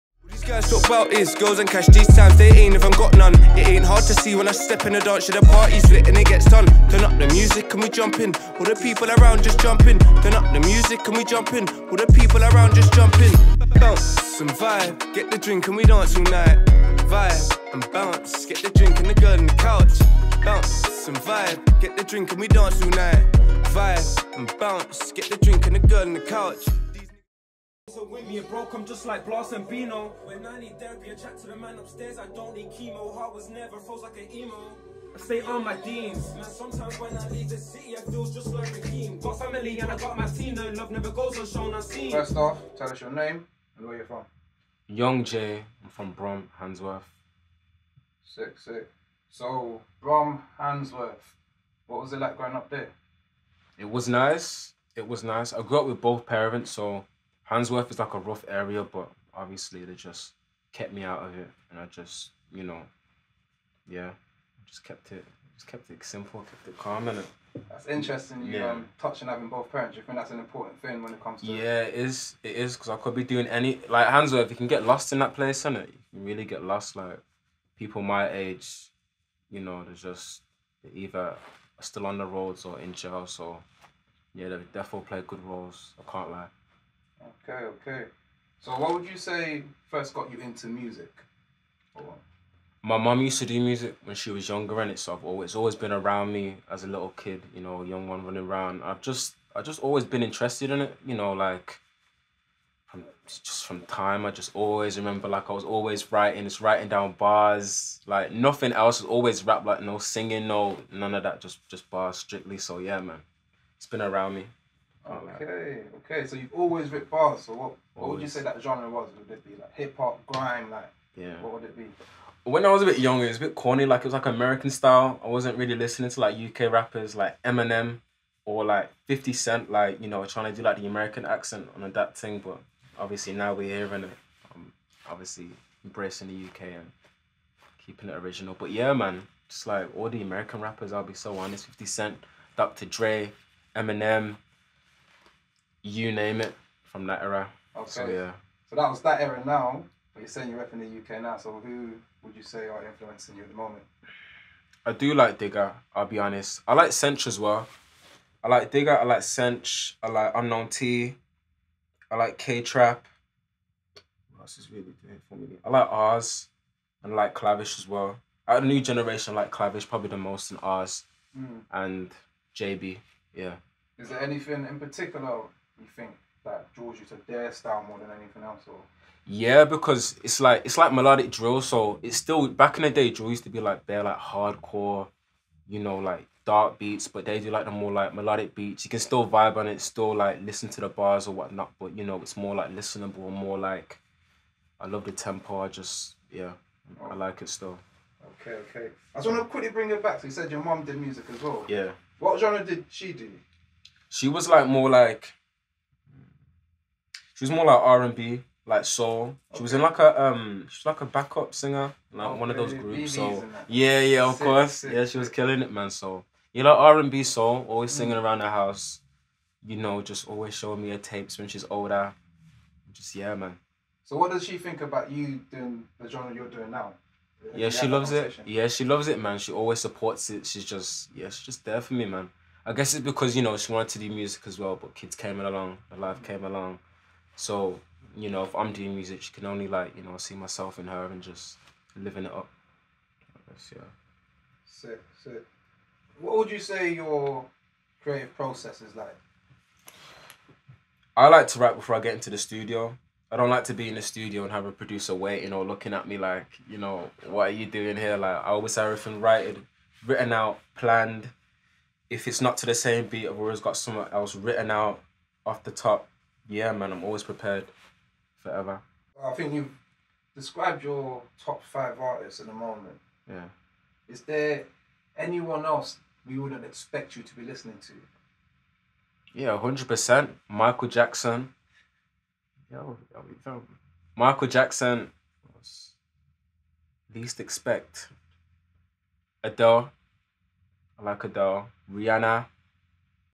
Young J - Growing up in Hansworth ||Having both parents || Getting into music & more..
Follow Young J on Instagram: @youngjmusicuk

Filmed || Edited by Mr Sols
Follow Mr Sols journey on Instagram: